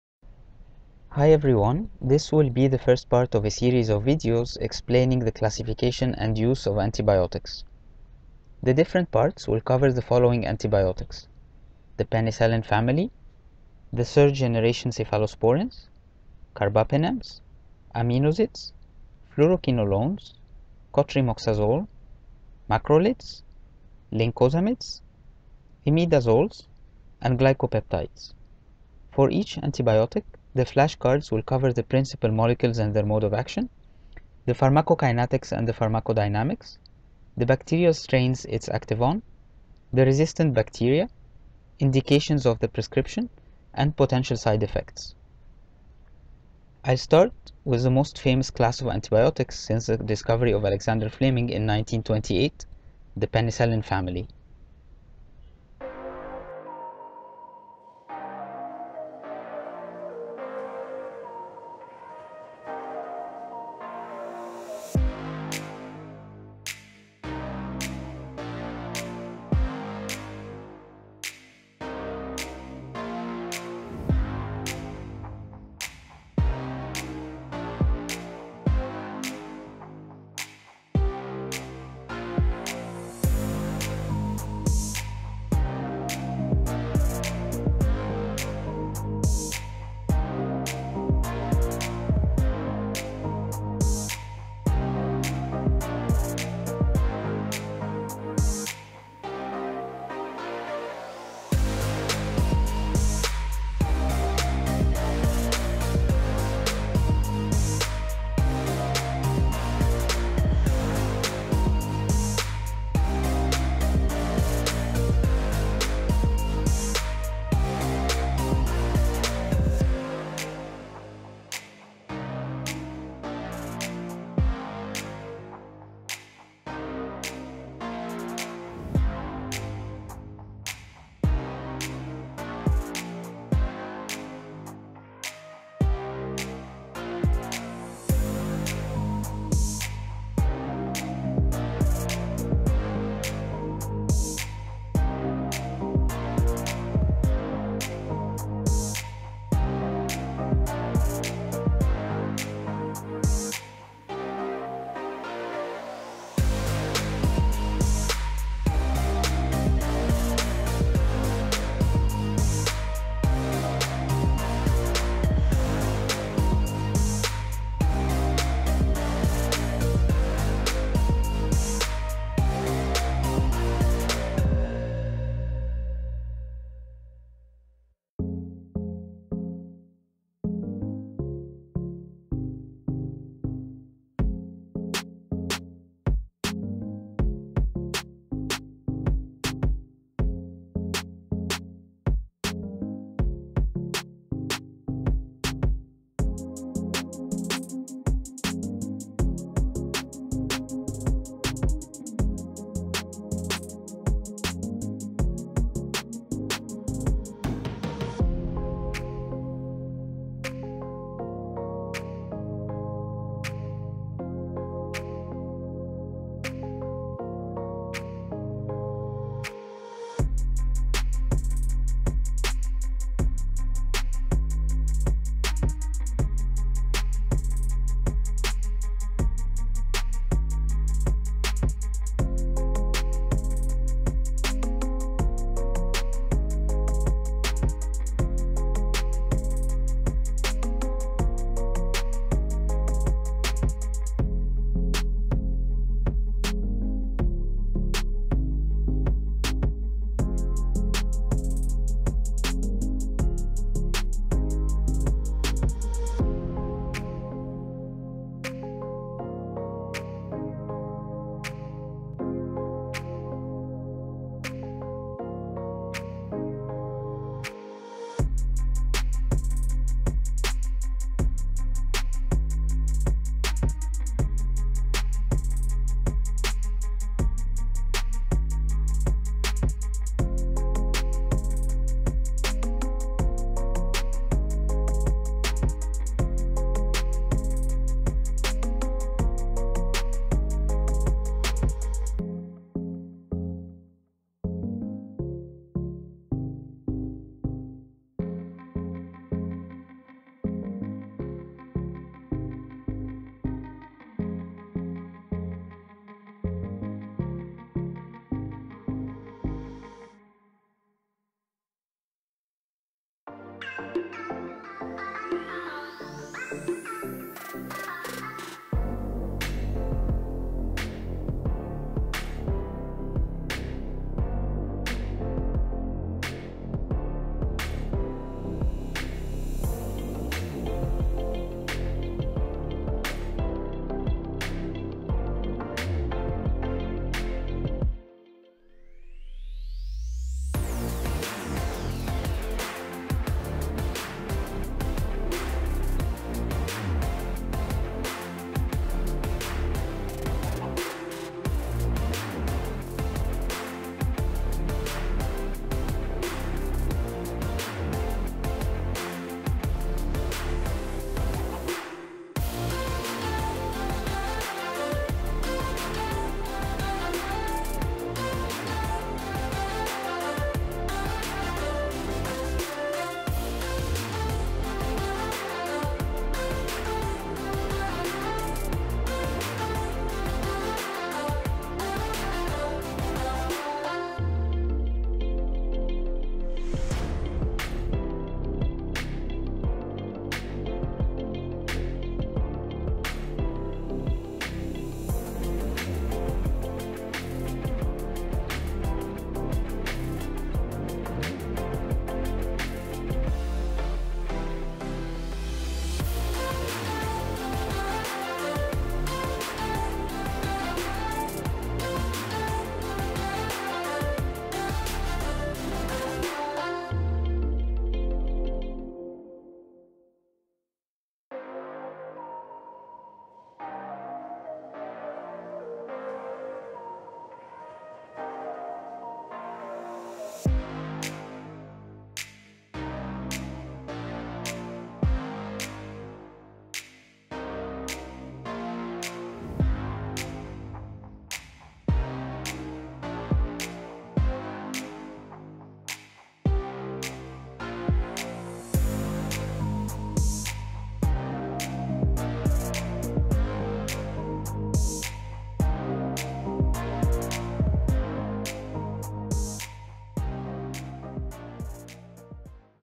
Antibiotics Flashcards | PART 1: PENICILLIN CLASS (from ECN-PILLY 2020)
Antibiotics are hard to memorize for medical students. I designed these flashcards to help myself and other students with the important aspects to know about each antibiotic class. The information in these flashcards are based on the French Medical school reference book (ECN-PILLY 2020).

 ---PS: The shapes and color of bacteria matches their morphology and Gram stain!
---CLARIFICATION: in minute 5:22 the flashcard says that oral penicillin M is useless, this might not be accurate, but its use is to be avoided due to its poor bioavailability as indicated later in the subsequent panels of the card

Let me know if you like this format so that I might make changes to the next flash cards to be uploaded!